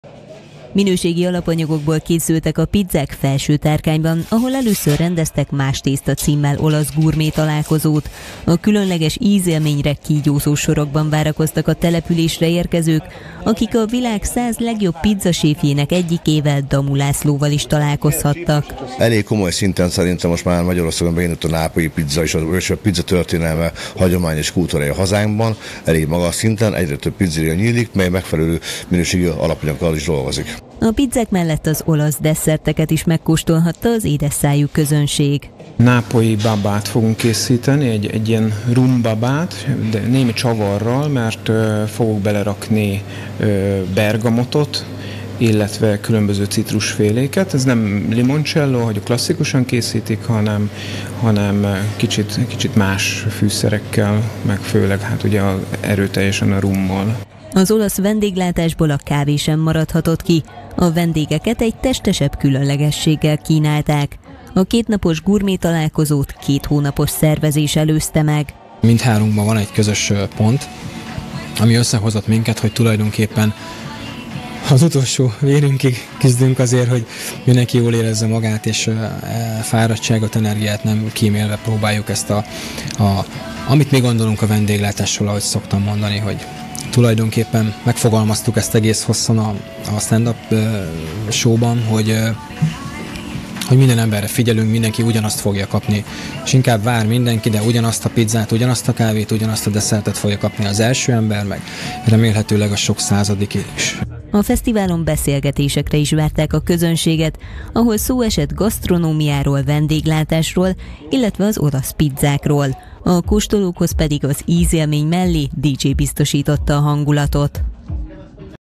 Más-Tészta: A vendéglátás legjavát kívánták bemutatni Felsőtárkányban - 2024.10.14.
Olasz pizza, desszertkülönlegességek és kávéspecialitások vártak a Felsőtárkányba érkezőkre hétvégén. A Más-Tészta elnevezésű gourmet találkozón DJ szórakoztatta a közönséget.